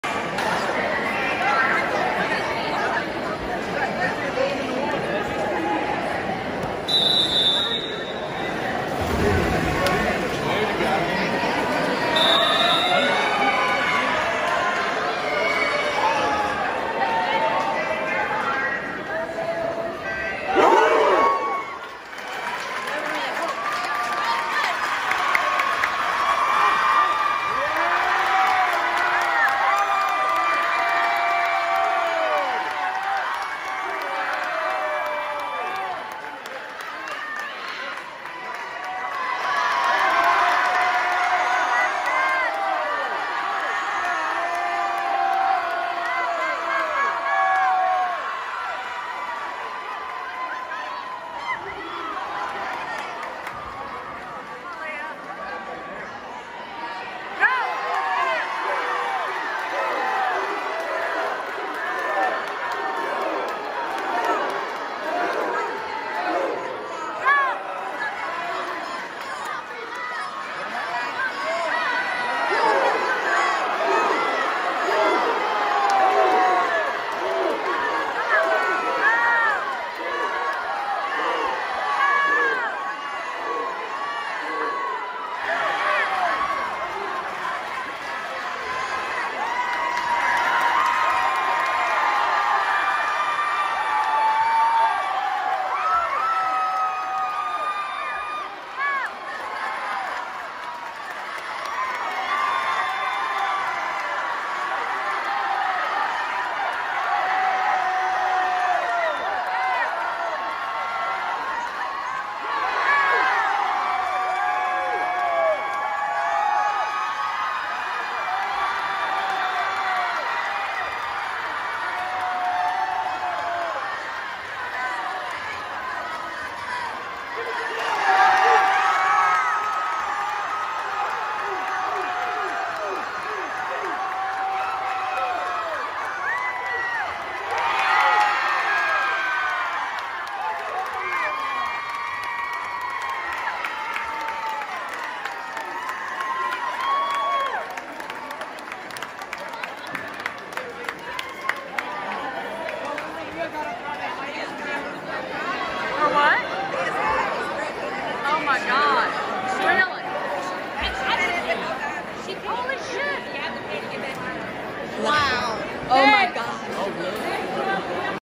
2021 200 medley relay
2021 LHSAA Division 3 finals. Leeas goggles broke on the block for Breaststroke. 11/18/2021